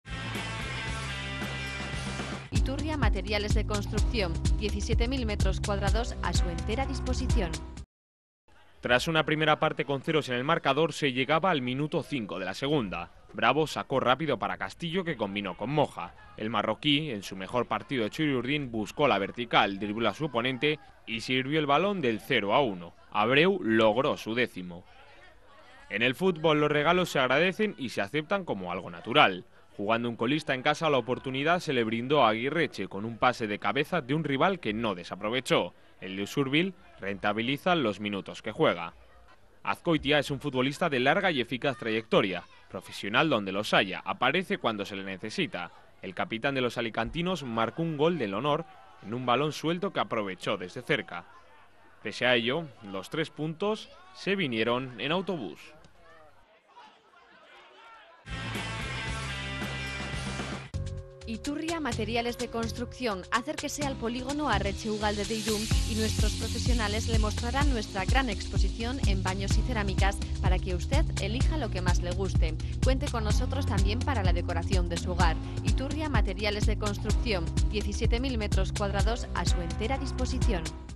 LOS GOLES DEL DIA - Alicante Real Sociedad
Los goles del partido disputado en el Rico Pérez entre Alicante y Real Sociedad